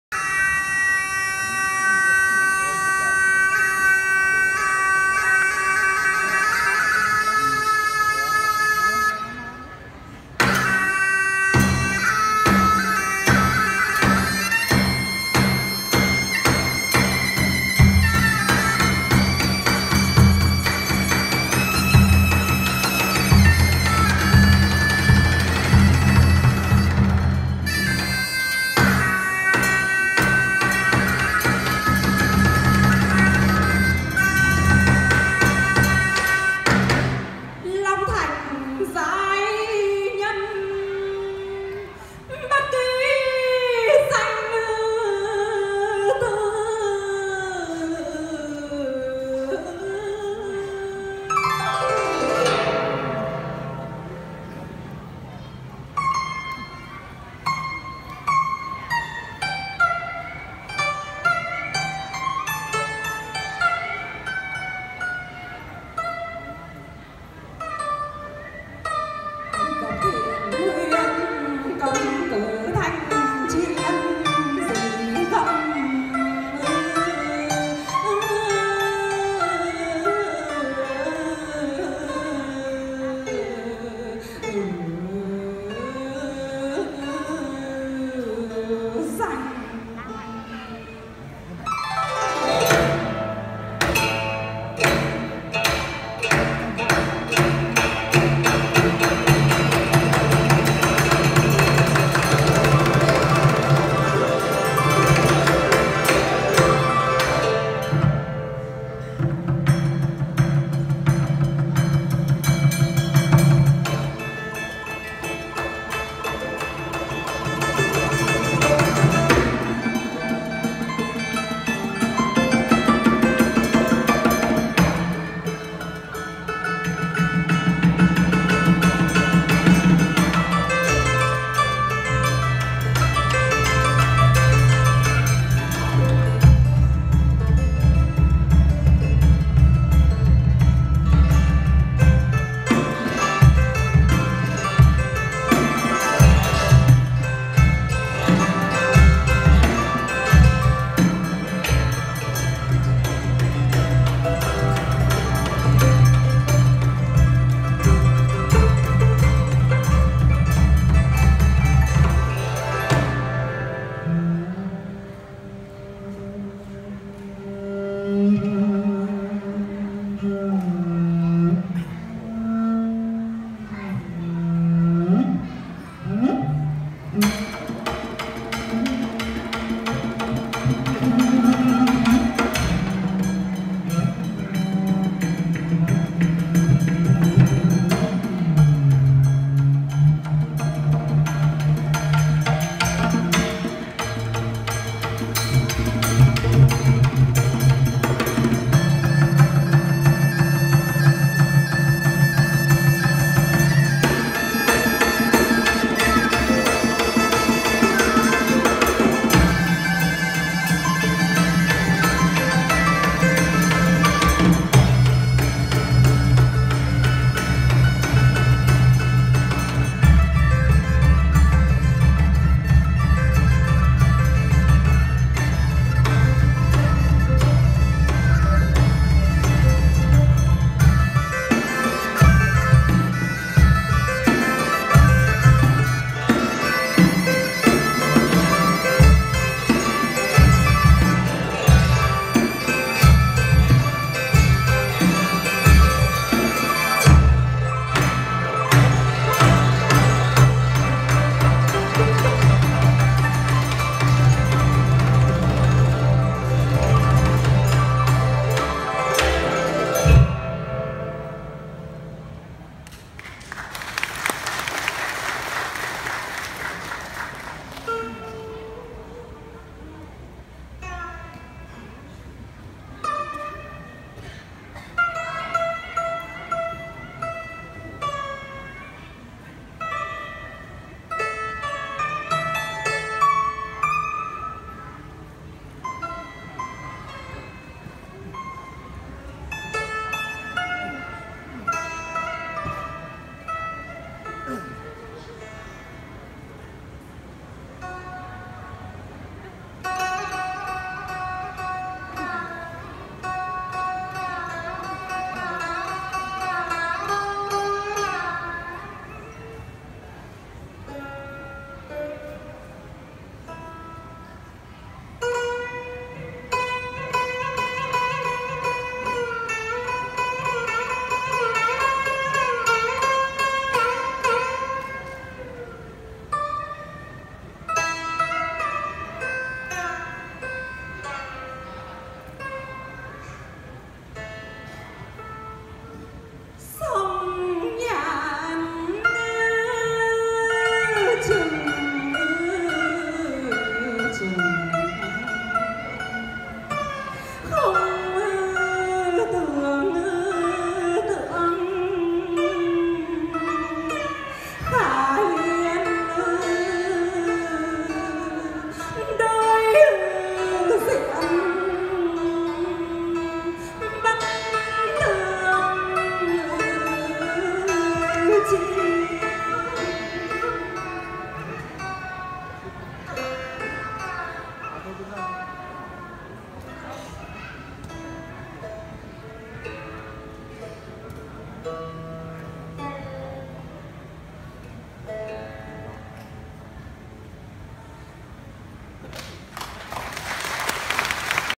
Độc tấu đàn thập lục. Biểu diễn: Tố Như
Nhà hát múa rối Thăng Long
liên hoan độc tấu và hòa tấu nhạc cụ truyền thống 2017